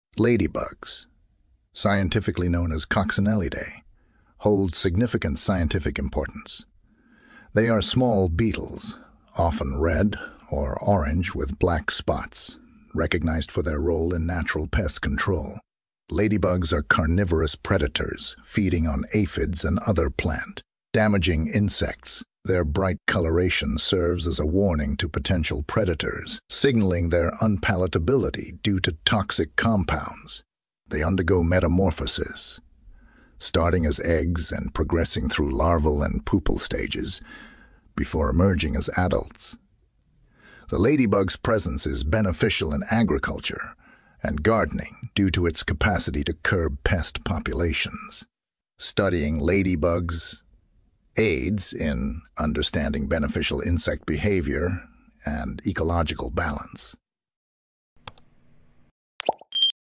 The Marvelous World of Ladybugs: Nature's Tiny Garden Heroes / Miraculous
Ladybugs, scientifically known as Coccinellidae, hold significant scientific importance. They are small beetles, often red or orange with black spots, recognized for their role in natural pest control. Ladybugs are carnivorous predators, feeding on aphids and other plant-damaging insects. Their bright coloration serves as a warning to potential predators, signaling their unpalatability due to toxic compounds. They undergo metamorphosis, starting as eggs and progressing through larval and pupal stages before emerging as adults. The ladybug's presence is beneficial in agriculture and gardening due to its capacity to curb pest populations. Studying ladybugs aids in understanding beneficial insect behavior and ecological balance.